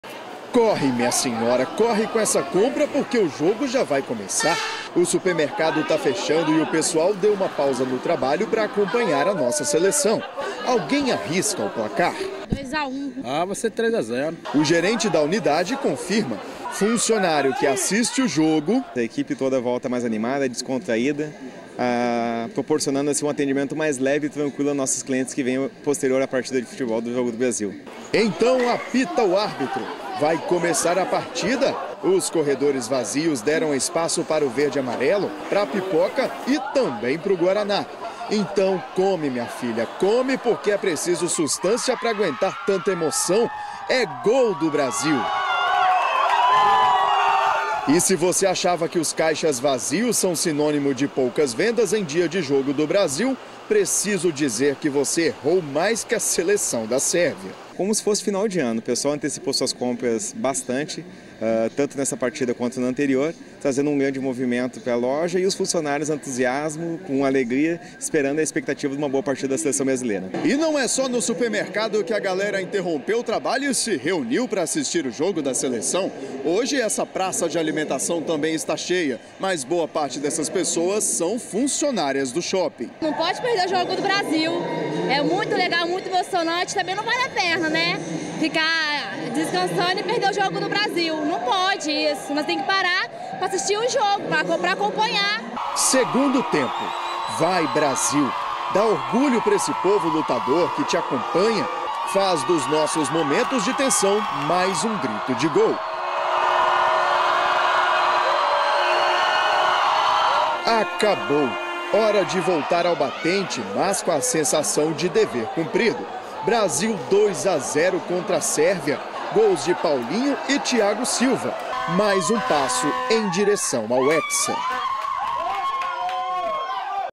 TN2: Torcida organizada para assistir a vitória da seleção brasileira
O Brasil precisava apenas de um empate para conquistar uma vaga na próxima fase.